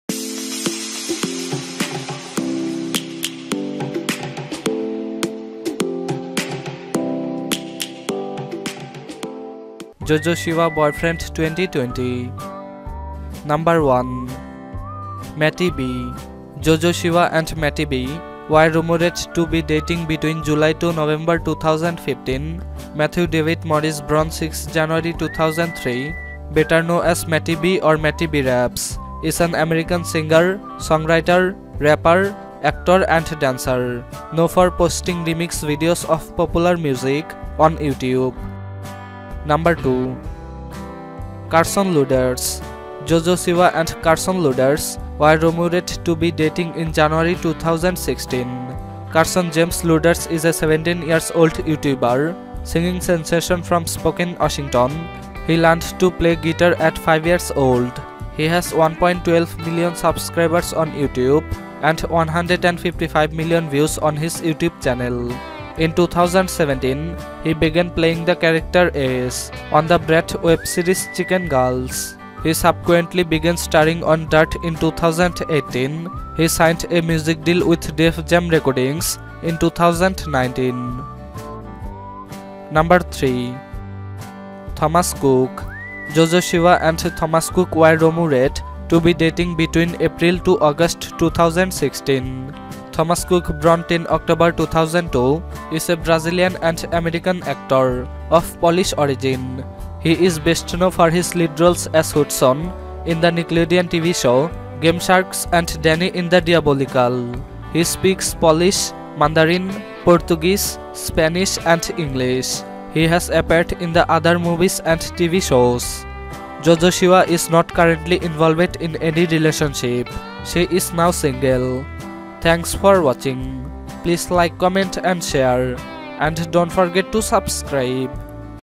Jojo Siwa Boyfriend 2020 || Jojo Siwa New Boyfriend
MattyB
Jojo Siwa and MattyB were rumoured to be dating between July to November 2015. Matthew David Morris born 6 January 2003 better known as MattyB or MattyBRaps, is an American singer, songwriter, rapper, actor and dancer known for posting remix videos of popular music on YouTube.
Carson Lueders
Jojo Siwa AndCarson Lueders were rumoured to be dating in January 2016. Carson James Lueders is a 17 year old Youtuber singing sensation from Spokane, Washington. He learned to play the guitar at 5 years old. He has 1.12 million subscribers on Youtube and 155 million views on his Youtube channel. In 2017, he began playing the character Ace on the Brat web series Chicken Girls. He subsequently began starring on Dirt in 2018. He signed a music deal with Def Jam Recordings in 2019.
Thomas Kuc
Jojo Siwa and Thomas Kuc were rumoured to be dating between April to August 2016. Thomas Kuc, born 10 October 2002, is a Brazilian-American actor of Polish origin. He is best known for his lead role as Hudson in the Nickelodeon TV show Game Shakers and Danny in The Diabolical. He speaks Polish, Mandarin, Portuguese, Spanish and English. He has appeared in other movies and TV shows.
Jojo Siwa is not currently involved in any relationship. She is now singel.